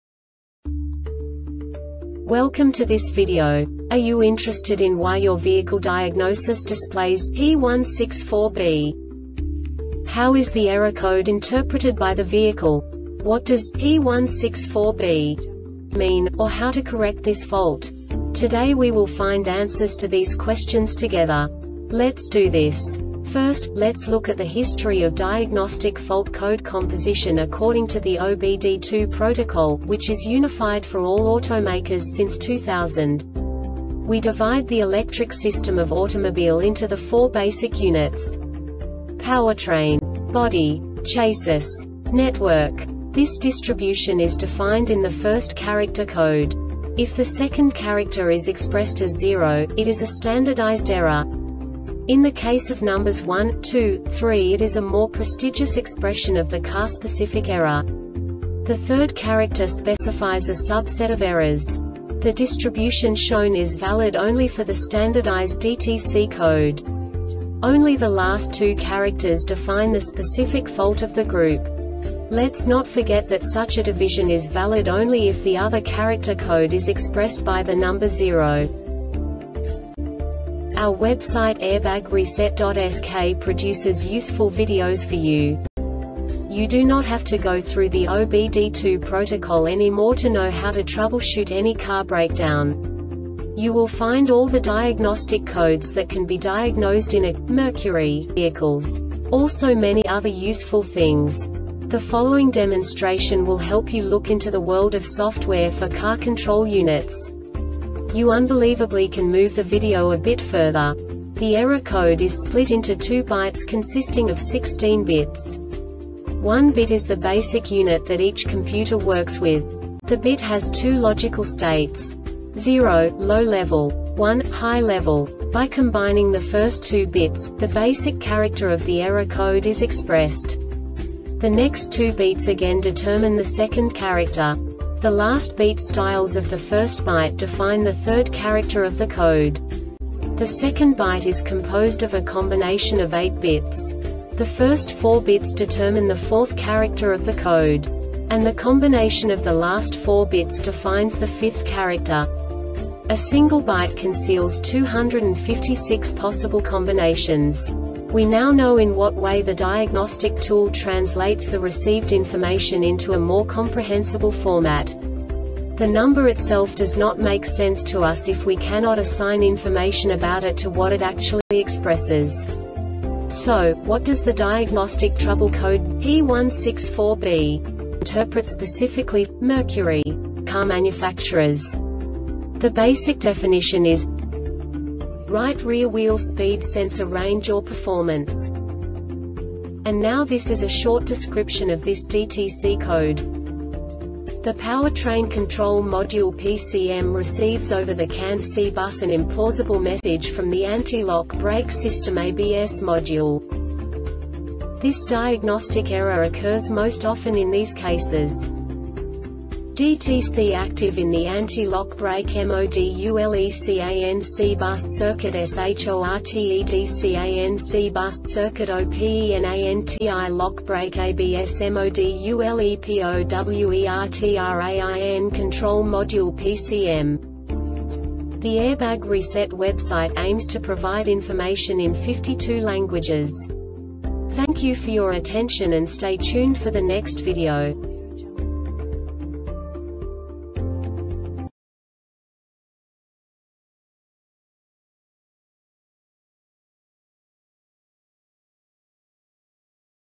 DTC Mercury P164B Short Explanation
Contents:
0:21 Basic DTC analysis according to OBD2 protocol standard.
1:48 Insight into programming
2:58 A diagnostically understandable description of the Diagnostic Trouble Code
3:05 Basic error definition

Make
Mercury
Code
P164B
Definition
Right Rear Wheel Speed Sensor Range or Performance
Description